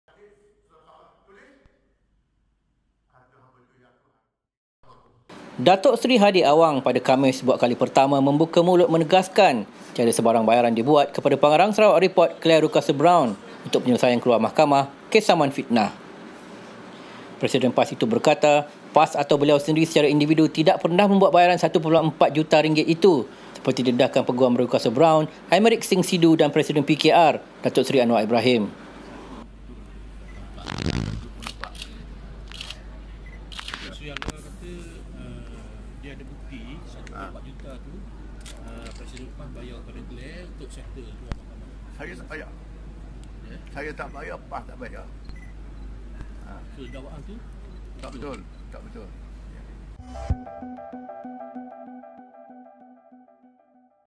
'Saya, PAS tak bayar Rewcastle-Brown' - Hadi Awang
Datuk Seri Hadi Awang pada Khamis buat kali pertama membuka mulut menegaskan tiada sebarang bayaran dibuat kepada Pengarang Sarawak Report, Clare Rewcastle-Brown, untuk penyelesaian luar mahkamah kes saman fitnah.